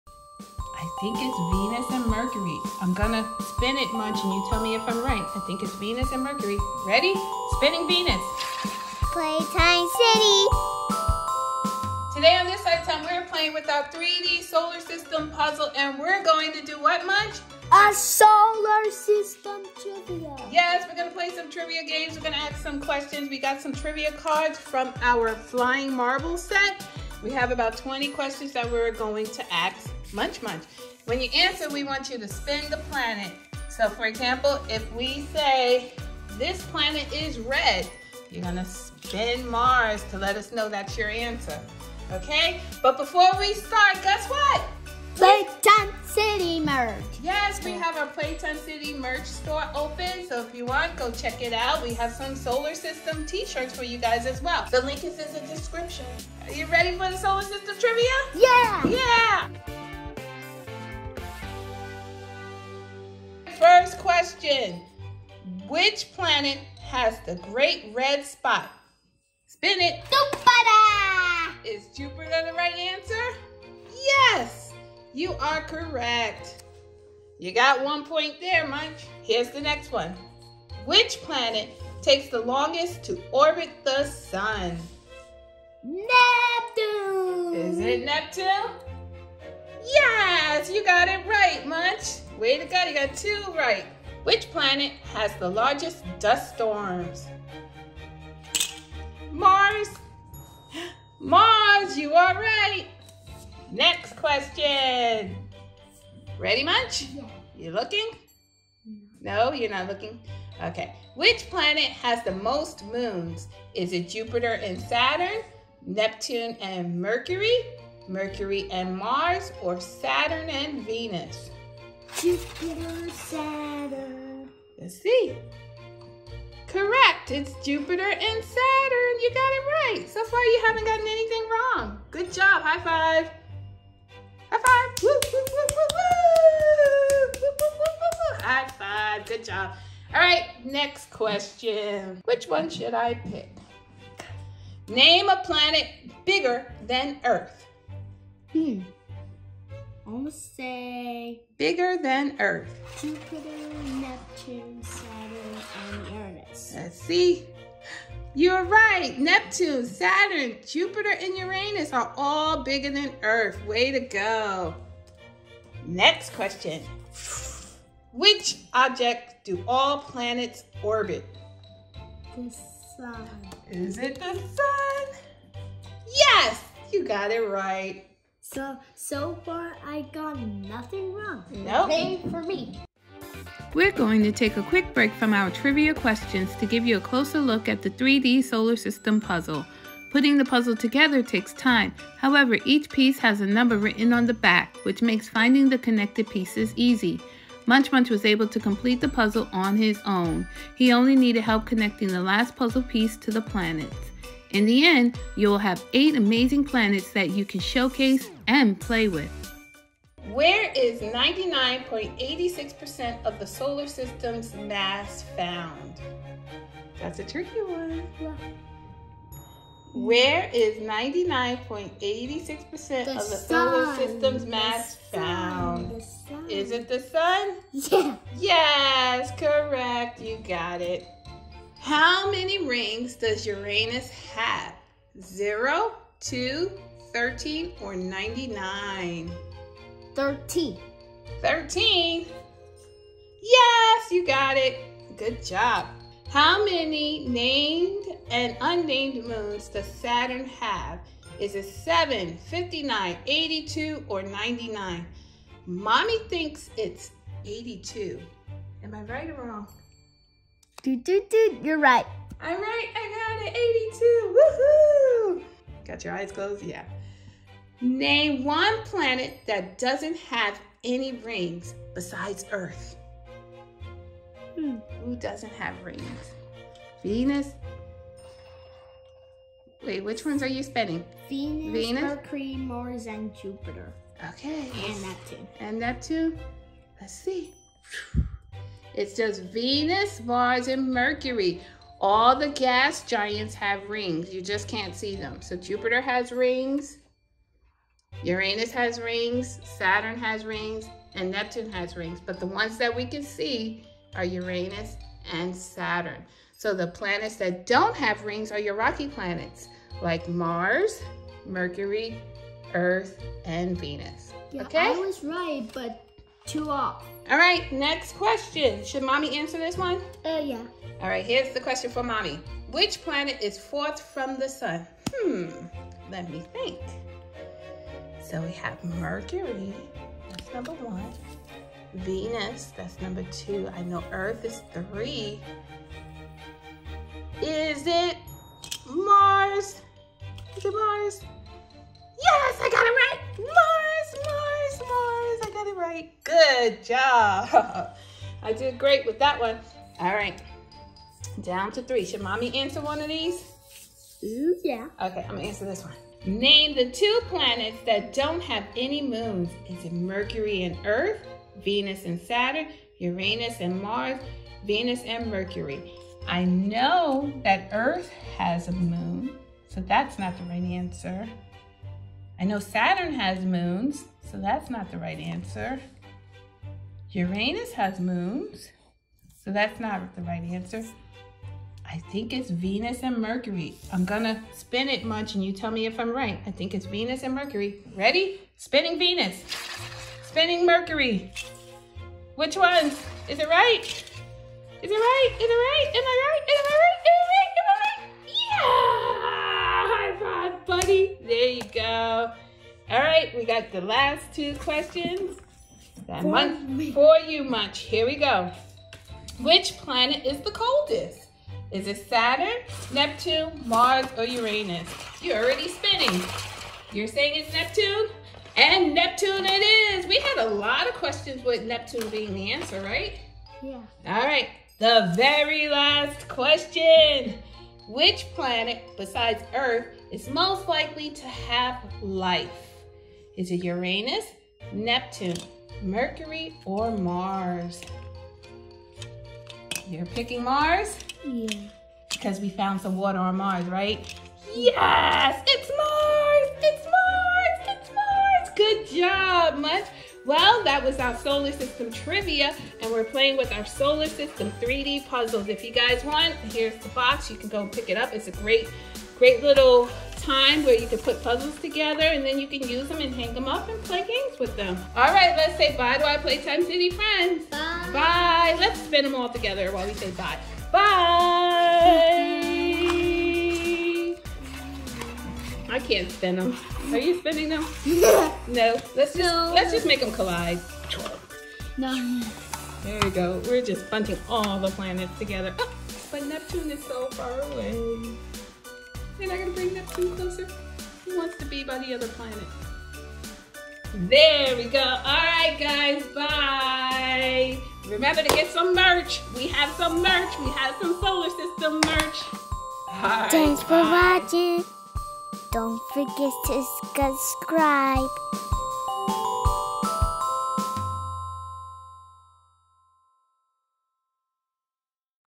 Solar System TRIVIA! - How much do you know about the planets? Fun with our 3D Solar System Puzzle
We’re playing with our 3D Solar System puzzle and answering trivia questions about the planets. Fun facts about the solar system planets